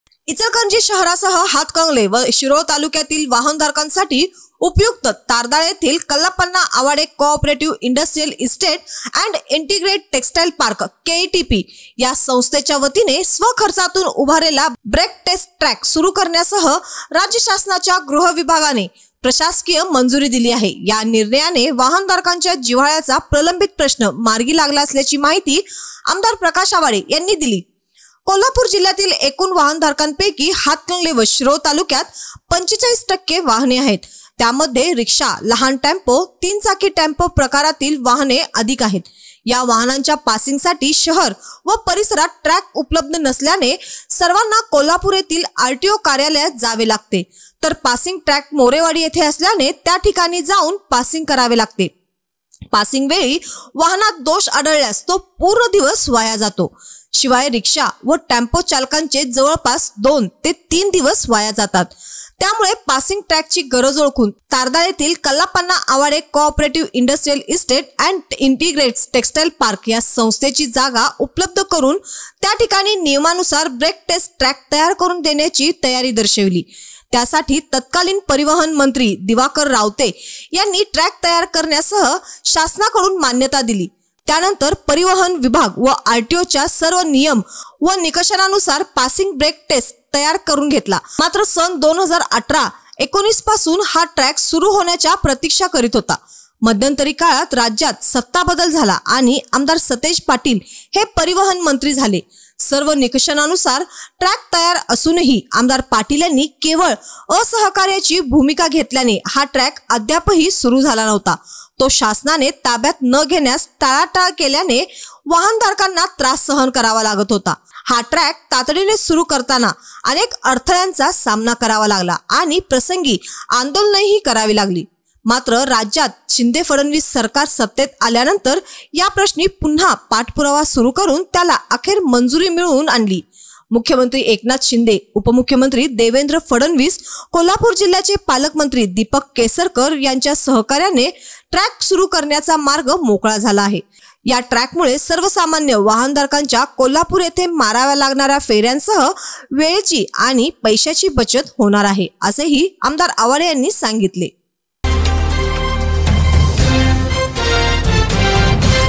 ब्रेक टेस्ट ट्रॅकला मान्यता; आमदार प्रकाश आवाडे यांच्या प्रयत्नांना यश
रिक्षा, टेम्पो, मिनी ट्रॅव्हल्स, मिनी ट्रक धारकांना दिलासा तारदाळ मध्ये होणार पासिंग